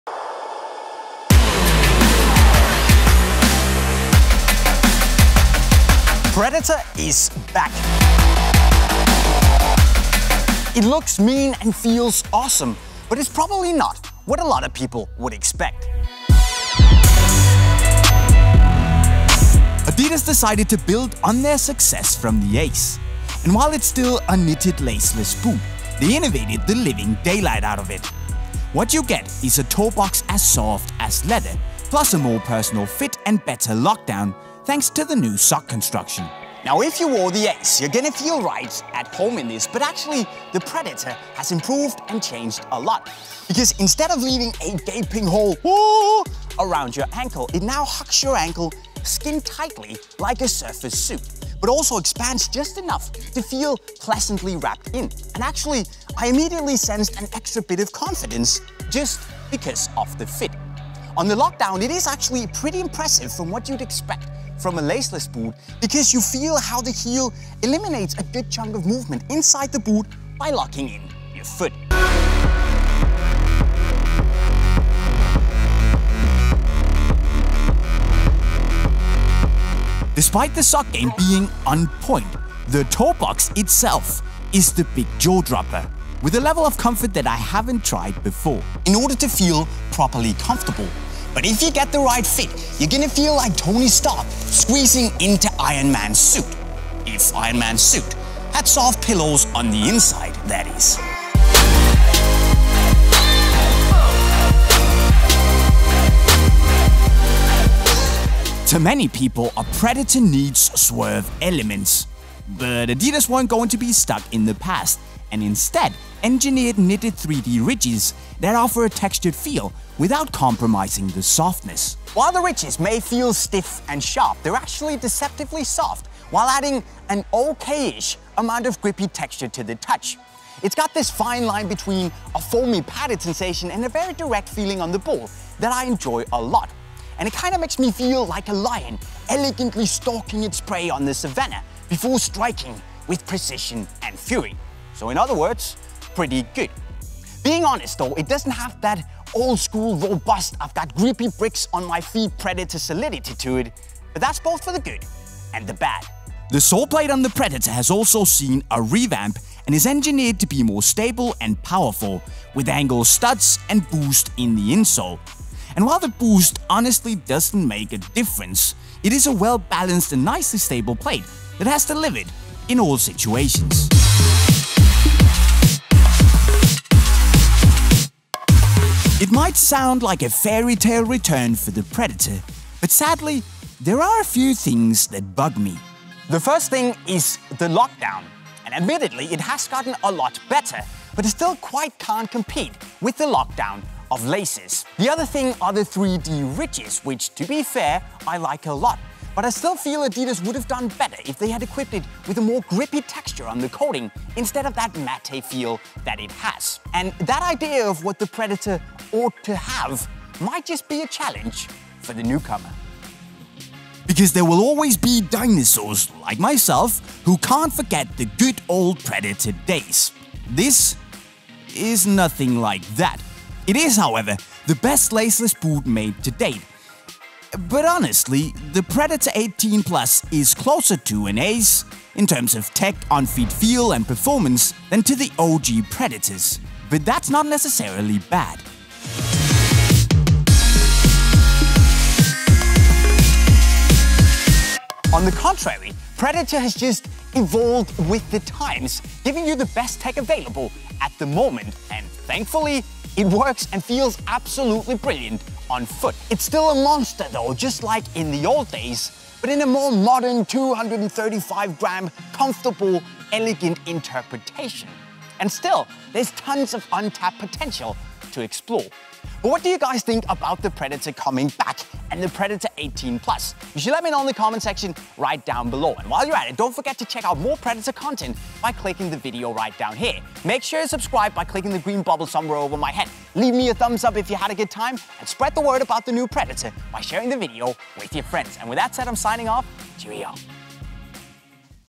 ADIDAS PREDATOR 18+ REVIEW
adidas Predator 18+ football boots. The adidas Predator is back - after 2,5 years away from the main stage, adidas have brought back their most iconic football boots silo. The Predator 18+ is here to give you the perfect fit and masterful control thanks to a newly engineered Primeknit upper, a Purecut sock and a super soft toebox with knitted 3D ridges to make the adidas Predator 18+ football boots a brilliant cocktail for the midfielders who control the game. The adidas Predator 18+ will be worn by elegant maestros such as Paul Pogba and Mesut Özil and will bring back the iconic football boots silo that was famously introduced in 1994 by adidas. Being a major part of football boot history, the adidas Predator is now back in its newest form, ditching the rubber elements, the tongue and the k-leather toebox but instead giving you a laceless football boot for maximum control on the ball.

GET THE ADIDAS PREDATOR 18+ FOOTBALL BOOTS RIGHT HERE:

With the new adidas Predator 18+ football boots, adidas are aiming to give you the best, most personal fit. Gone is the loose sock around your ankle from the adidas ACE17+ PURECONTROL and instead you get at completely personal fit in the Predator 18+ around the ankle. It will also provide a much better lockdown on your foot thanks to the tighter heel counter and forged knit around the midfoot, and in general the Predator 18+ football boots will give you both comfort, fit and a masterful touch on the ball. In todays video JayMike will tell you how it performs on the pitch and break down to you why you should spend the 300€ on the brand new adidas Predator 18+ football boots.